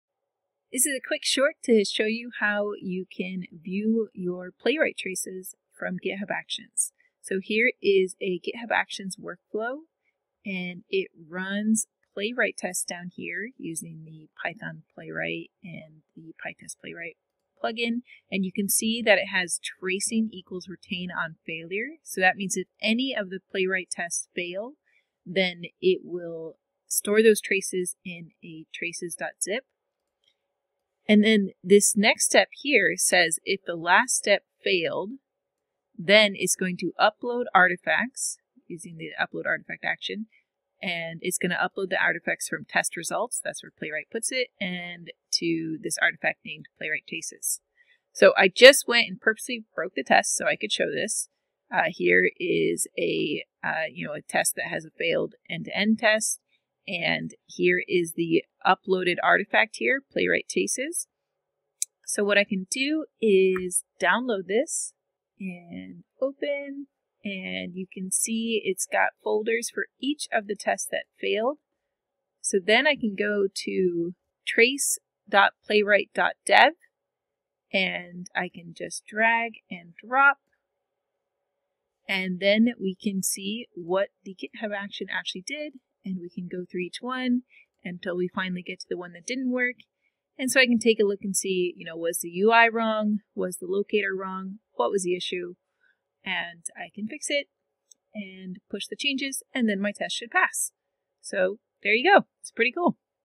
Viewing Playwright Traces from GitHub Actions Artifacts
A quick demonstration of using GitHub actions to run Playwright tests (using Python and Pytest), save traces for failed tests, upload those artifacts, and view them in Playwright Trace Viewer.